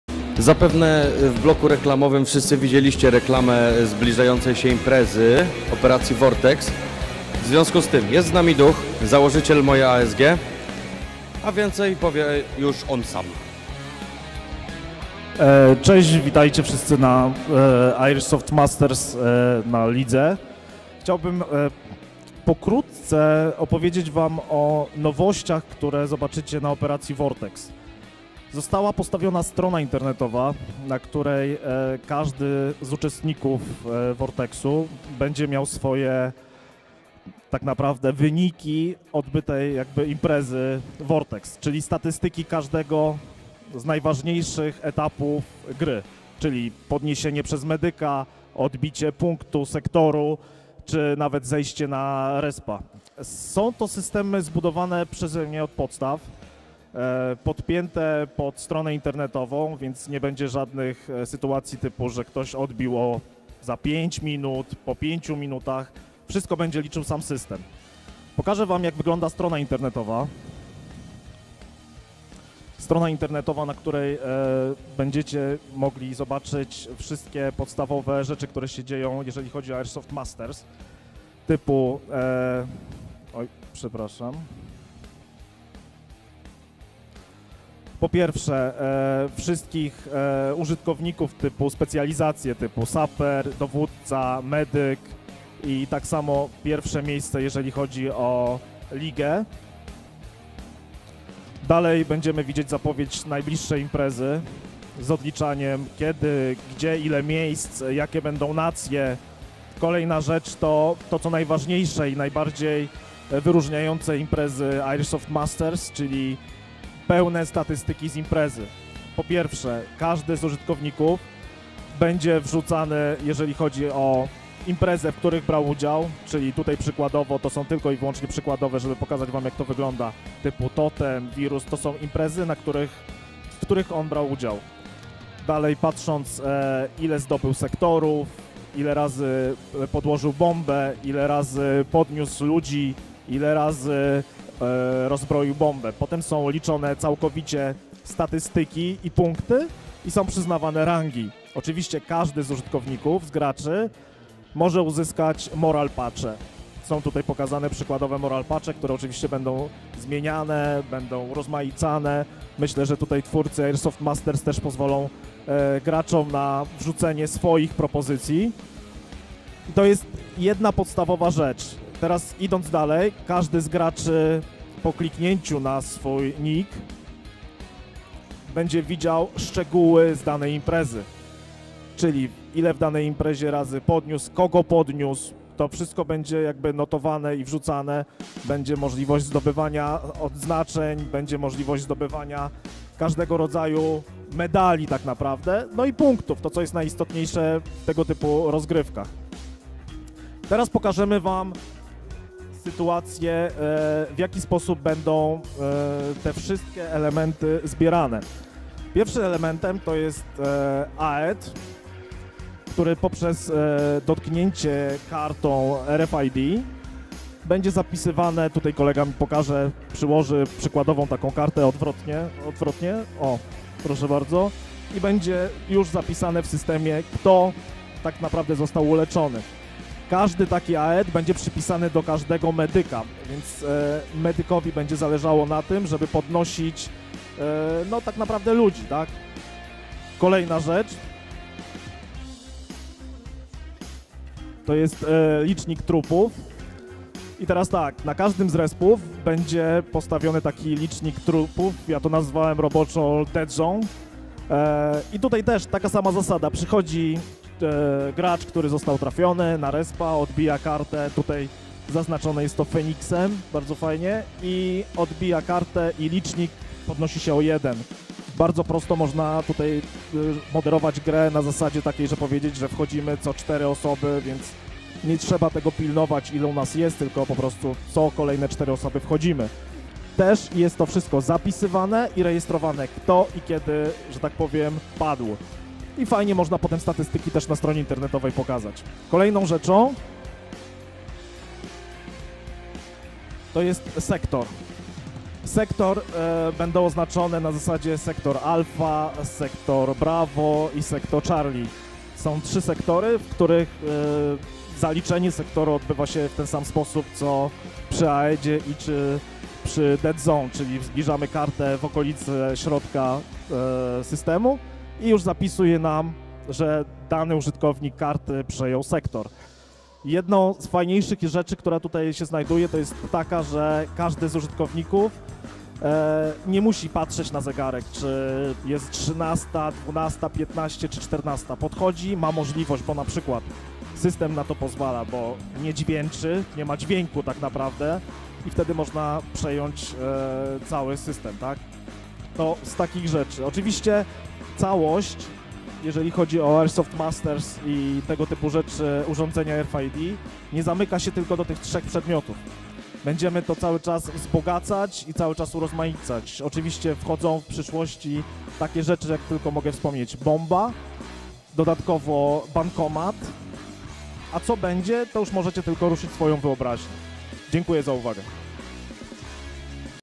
DAL LIVE - Prezentacja systemów RFID - OPERACJA VORTEX
Podczas zawodów Dynamic Air League gościliśmy "Ducha" założyciela MojeASG, każdy Warszawski airsoftowiec powinien pamiętać ich strzelanki! Dzięki współpracy ASM i MojeASG tworzone są innowacyjne systemy, których użyjemy podczas Operacji Vortex, podczas transmisji Live Duch dość skrupulatnie i szczegółowo opisał zasadę działania, i zaprezentował system statystyk. Zobaczcie sami z czym będziecie mieli styczność podczas imprezy!